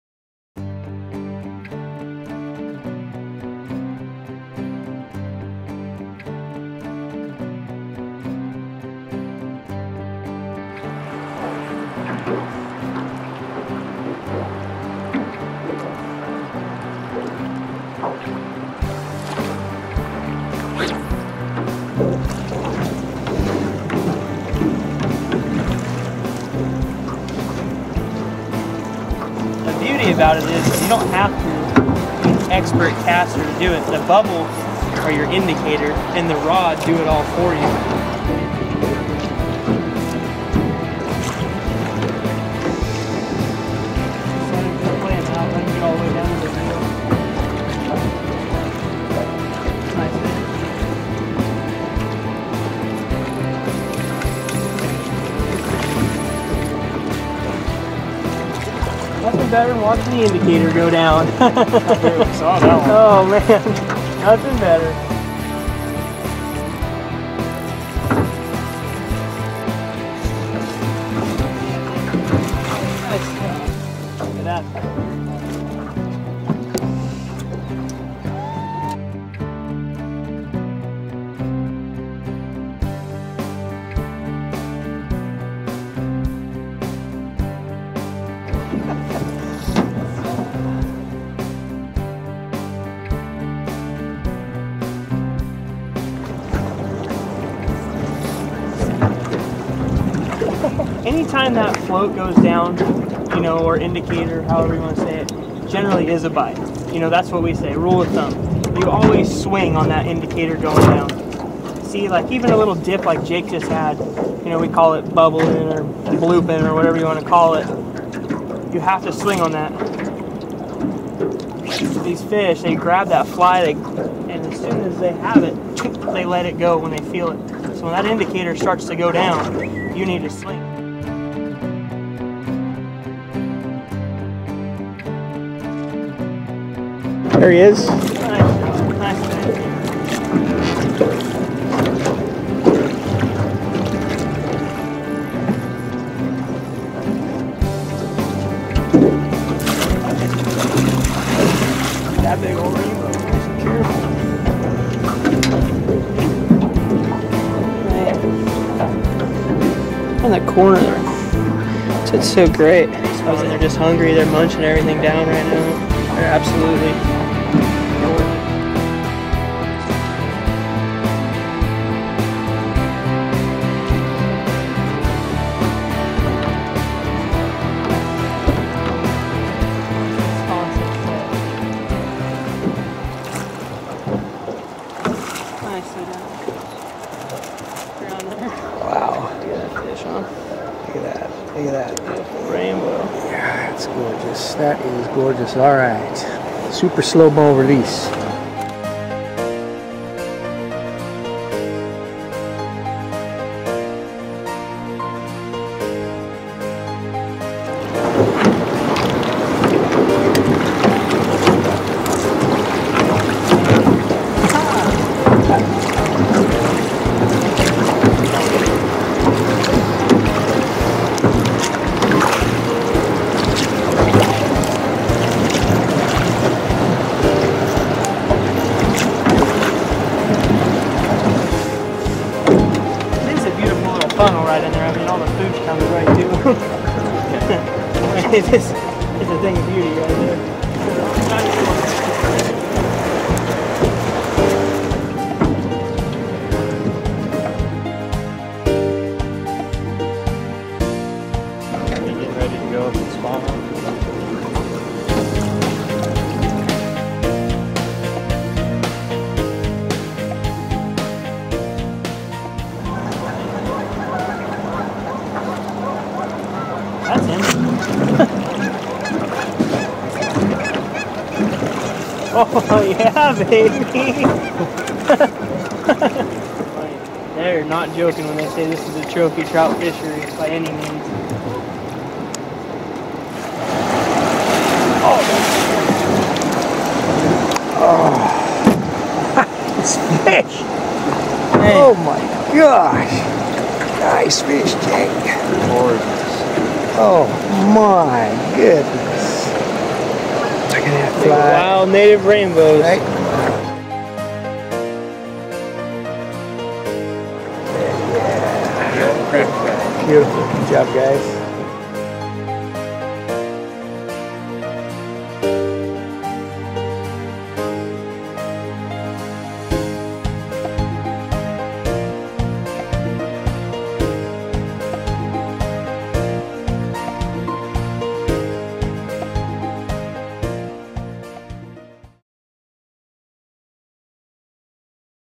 Fly Fishing for Wild Rainbow Trout
A favorite of Professional Guide Kevin Brock is Fly Fishing for Wild Rainbow Trout on Northern California's Sacramento River.

Join the KB Team for a float down this beautiful trophy fishing stream with gorgeous wild fish and natural river beauty. ALERT! When you see all this, plus KB's first-class fly-fishing boat and gear, you'll be opening your calendar to book a trip.